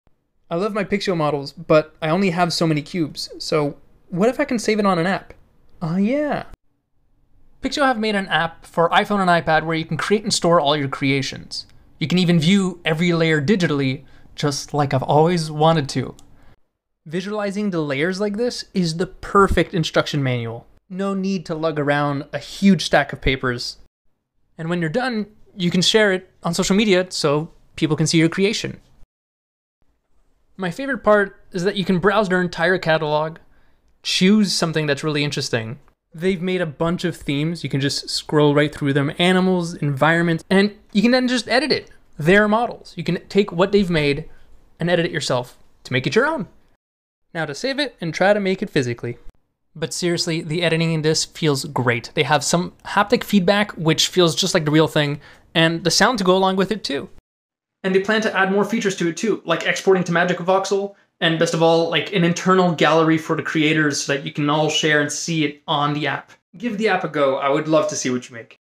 Pixio iOS app for voxel editing!
PIXIO have made an iOS app for voxel editing. i make things with their cubes all the time but im glad i can store it on my phone now. like my own instructions catalogue.

you should give it a go!